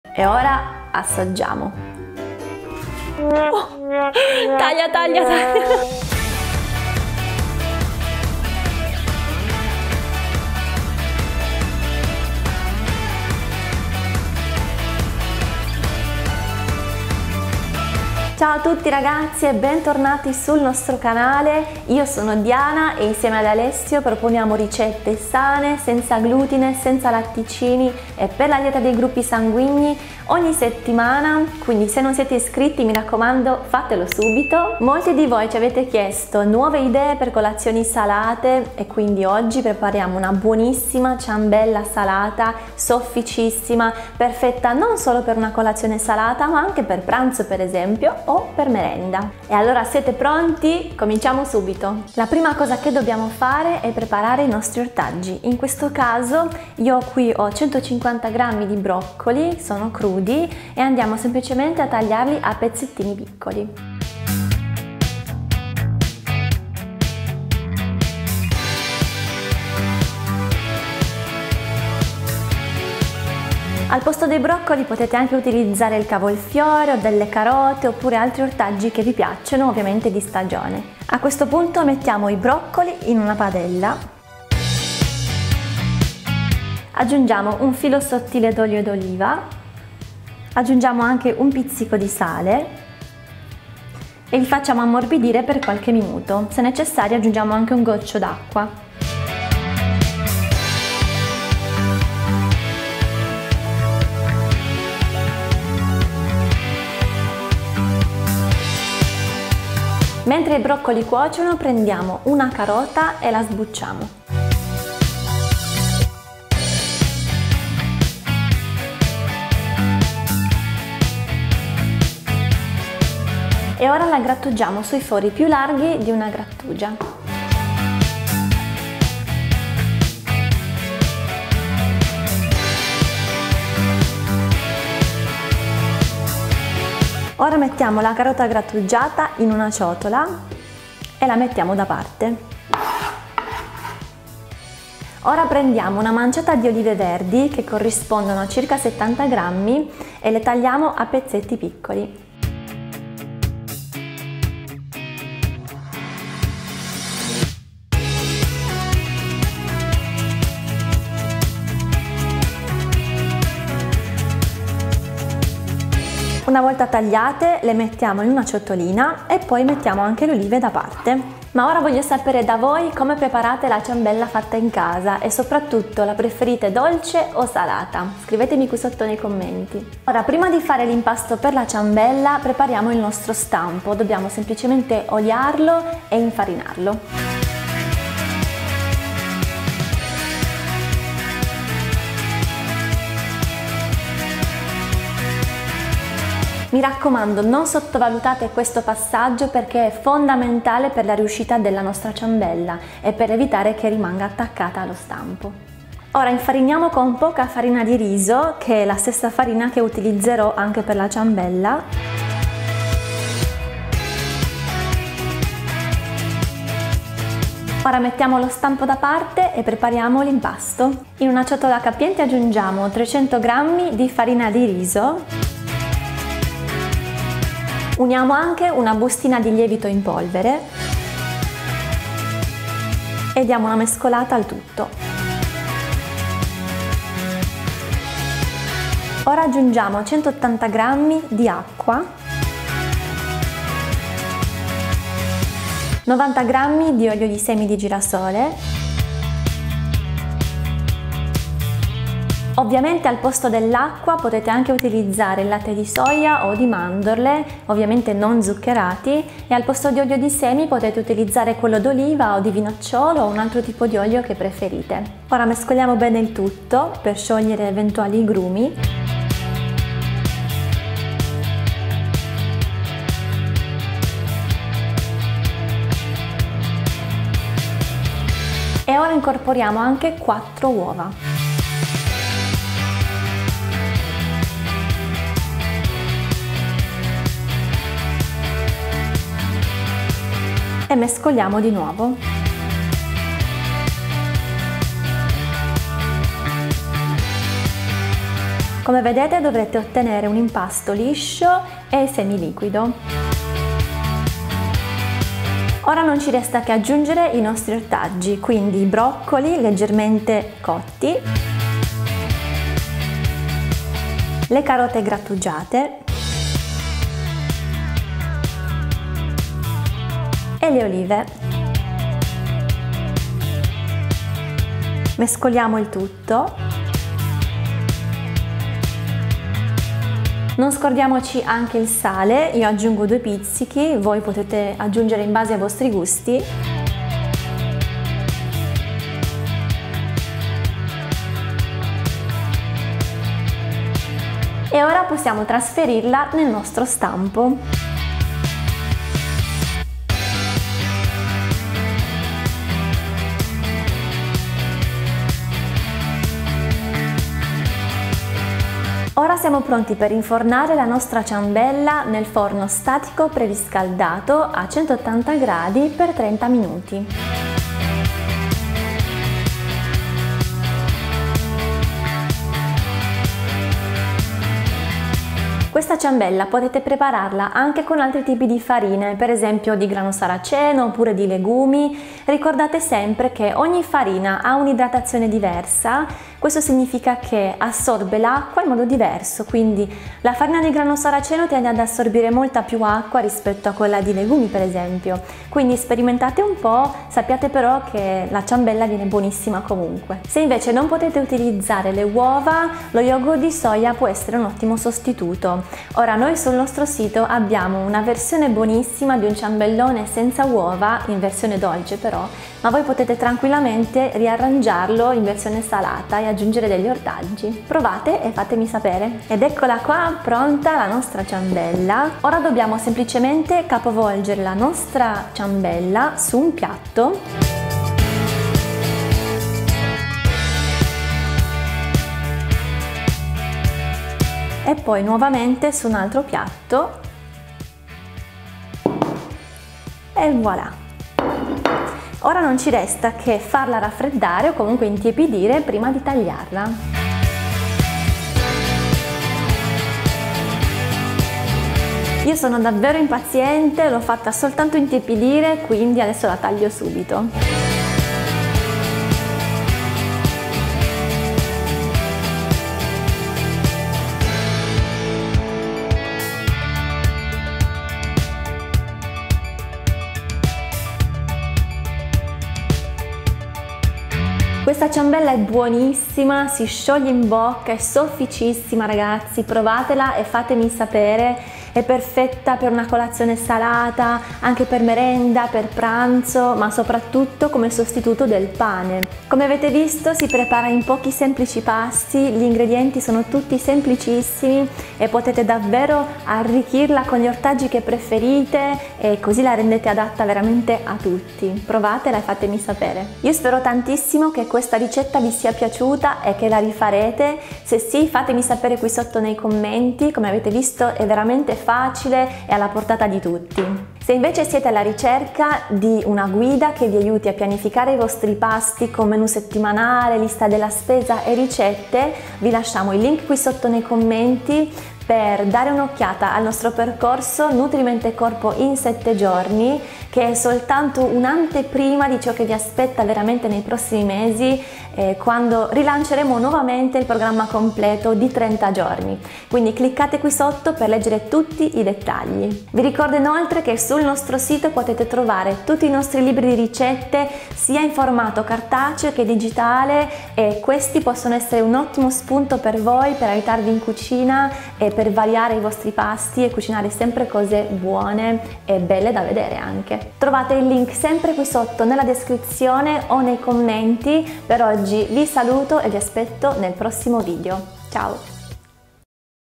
TI PRESENTO LA CIAMBELLA PIÙ SOFFICE CHE HAI MAI MANGIATO!
Non ci sono scuse!
Questa ciambella sofficissima devi ASSOLUTAMENTE provarla!
È davvero una bontà ed è talmente soffice e delicata che si scioglie in bocca 😮 La più soffice che hai mai mangiato!

Questa nostra versione è SENZA GLUTINE e SENZA LATTE, adatta ai gruppi sanguigni 0 e A.
Il gruppo B omettere le olive verdi e utilizzare un altro olio consentito al posto dell olio di girasole.
Il gruppo AB utilizzare un altro olio consentito al posto dell'olio di girasole.

Se vuoi organizzare la tua alimentazione e implementare nuove abitudini sane nella tua vita in 7 giorni allora questo fa per te: